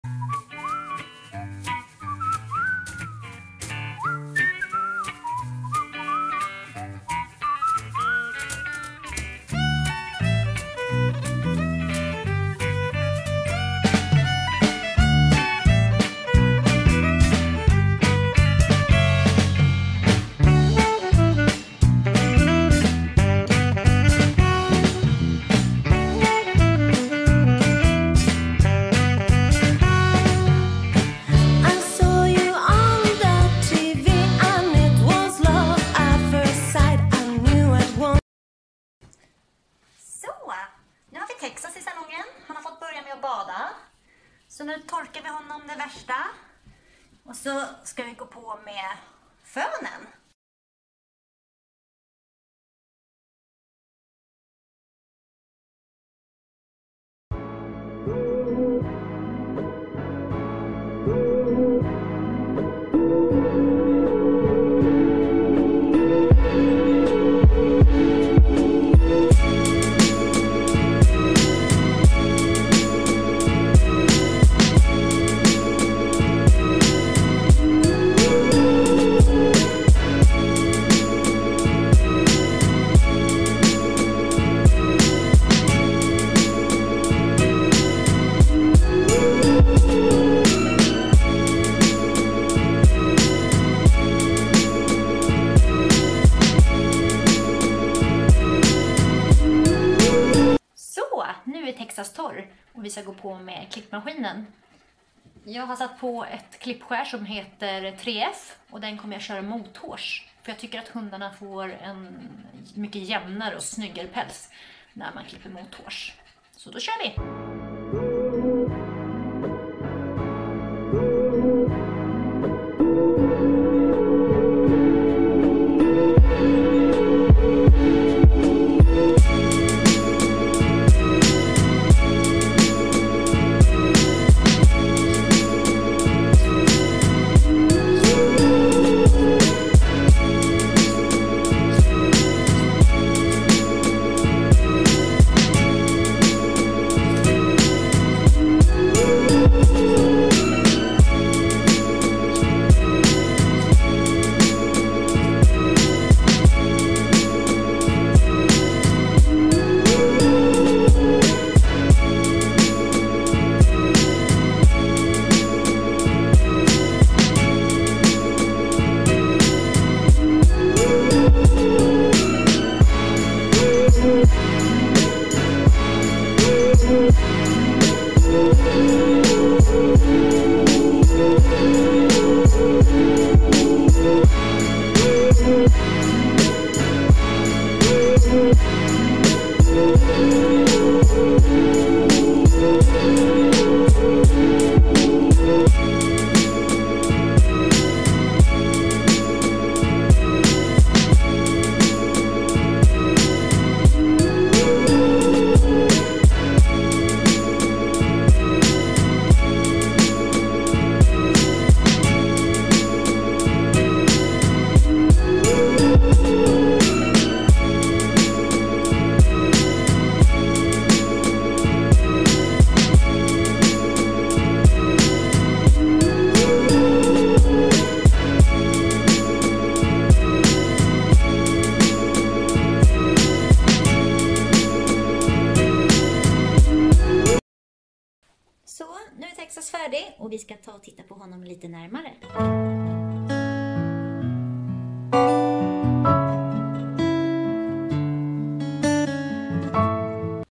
Klipper en Cockerpoo. Texas hos Fru Hast
En liten inblick i hundsalongen.